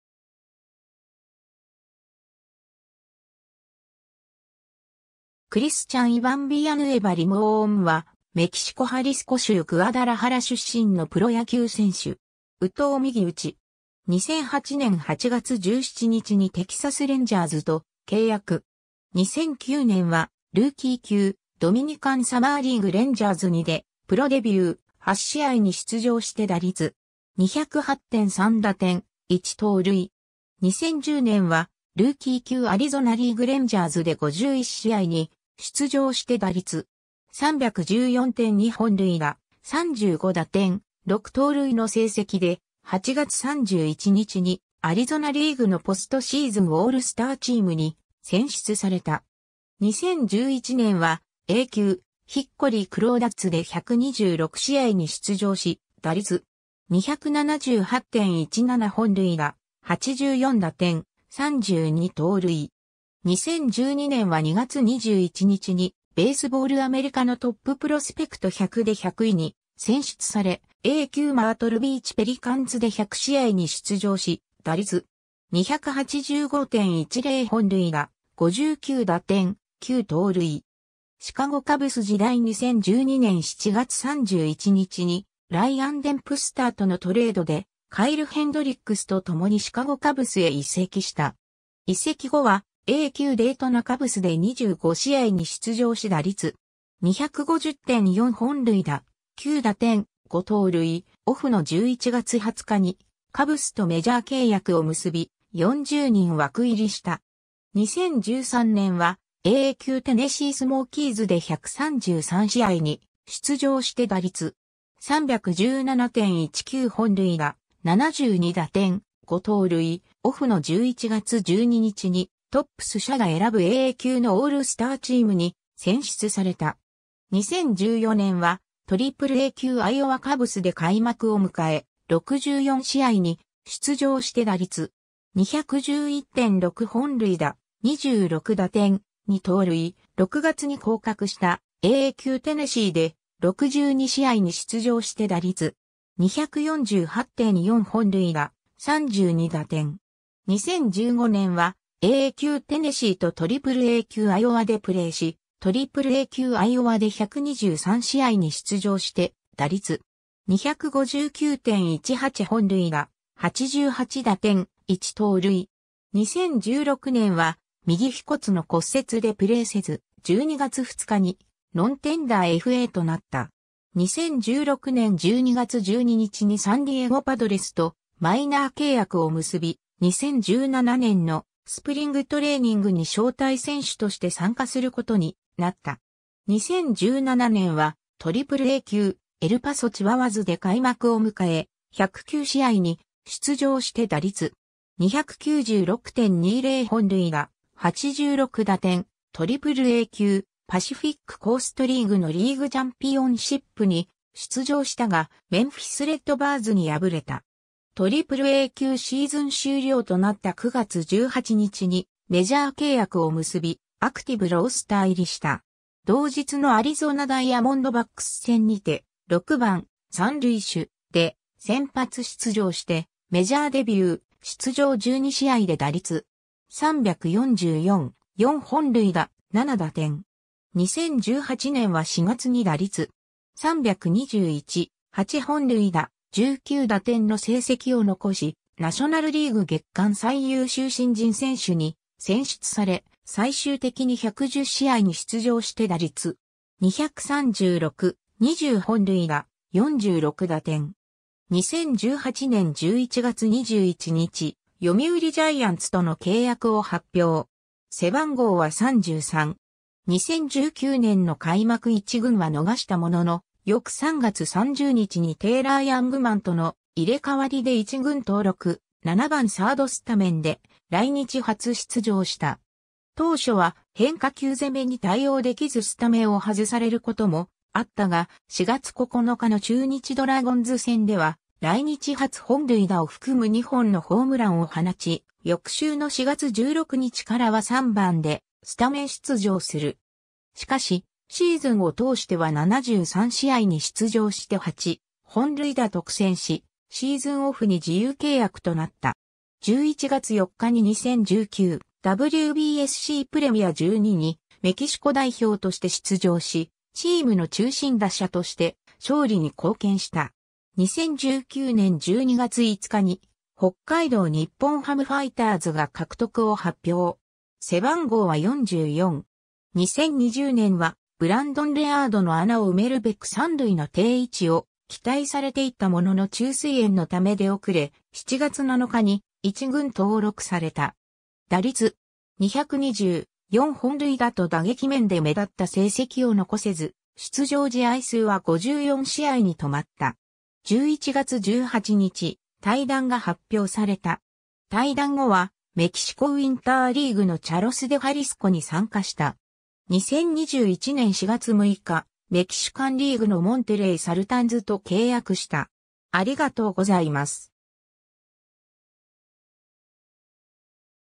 クリスチャン・ビヤヌエバ
モンテレイ・サルタンズの選手
メキシコ・グアダラハラ出身の人物
クリスチャン・イバン・ビヤヌエバ・リモーン（Christian Iván Villanueva Limón, 発音：vee-yan-WAY-vuh, 1991年6月19日 - ）は、メキシコ・ハリスコ州グアダラハラ出身のプロ野球選手（内野手）。
右投右打。
 2008年8月17日にテキサス・レンジャーズと契約。
 2009年はルーキー級ドミニカン・サマーリーグ・レンジャーズ2でプロデビュー、8試合に出場して打率.208・3打点・1盗塁。
 2010年はルーキー級アリゾナリーグ・レンジャーズで51試合に出場して打率.314・2本塁打・35打点・6盗塁の成績で、8月31日にアリゾナリーグのポストシーズン・オールスターチームに選出された。
 2011年はA級ヒッコリー・クロウダッズで126試合に出場し打率.278・17本塁打・84打点・32盗塁。
 2012年は2月21日にベースボール・アメリカのトッププロスペクト100で100位に選出され、A+級マートルビーチ・ペリカンズで100試合に出場し打率.285・10本塁打・59打点・9盗塁。
 シカゴ・カブス時代 (2013年3月10日) 2012年7月31日にライアン・デンプスターとのトレードで、カイル・ヘンドリックスと共にシカゴ・カブスへ移籍した。
移籍後はA+級デイトナ・カブスで25試合に出場し打率.250・4本塁打・9打点・5盗塁、オフの11月20日にカブスとメジャー契約を結び、40人枠入りした。
 2013年はAA級テネシー・スモーキーズで133試合に出場して打率.317・19本塁打・72打点・5盗塁、オフの11月12日にTopps社が選ぶAA級のオールスターチームに選出された。
 2014年はAAA級アイオワ・カブスで開幕を迎え、64試合に出場して打率.211・6本塁打・26打点・2盗塁、6月に降格したAA級テネシーで、62試合に出場して打率.248・4本塁打・32打点。
 2015年はAA級テネシーとAAA級アイオワでプレーし、AAA級アイオワで123試合に出場して打率.259・18本塁打・88打点・1盗塁。
 2016年は右腓骨の骨折でプレーせず、12月2日にノンテンダーFAとなった。
 2016年12月12日にサンディエゴ・パドレスとマイナー契約を結び、2017年のスプリングトレーニングに招待選手として参加することになった。
 2017年はAAA級エル・パソ・チワワズで開幕を迎え、109試合に出場して打率.296・20本塁打・86打点、AAA級パシフィック・コーストリーグのリーグチャンピオンシップに出場したが、メンフィス・レッドバーズに敗れた。
AAA級シーズン終了となった9月18日にメジャー契約を結び、アクティブ・ロースター入りした。
同日のアリゾナ・ダイヤモンドバックス戦にて「6番・三塁手」で先発出場してメジャーデビュー、出場12試合で打率.344、4本塁打、7打点。
 2018年は4月に打率.321、8本塁打、19打点の成績を残しナショナル・リーグ月間最優秀新人選手に選出され、最終的に110試合に出場して打率.236、20本塁打、46打点。
 2018年11月21日、読売ジャイアンツとの契約を発表。
 2019年の開幕一軍は逃したものの、翌3月30日にテイラー・ヤングマンとの入れ替わりで一軍登録、7番サードスタメンで来日初出場した。
当初は変化球攻めに対応出来ずスタメンを外されることもあったが、4月9日の中日ドラゴンズ戦では来日初本塁打を含む2本のホームランを放ち、翌週の4月16日からは3番でスタメン出場する。
しかし、シーズンを通しては73試合に出場して8本塁打と苦戦し、シーズンオフに自由契約となった。
11月4日に2019 WBSCプレミア12にメキシコ代表として出場し、チームの中心打者として勝利に貢献した。
 2019年12月5日に北海道日本ハムファイターズが獲得を発表。
 2020年はブランドン・レアードの穴を埋めるべく三塁の定位置を期待されていたものの虫垂炎の為出遅れ、7月7日に1軍登録された。
打率.220、4本塁打と打撃...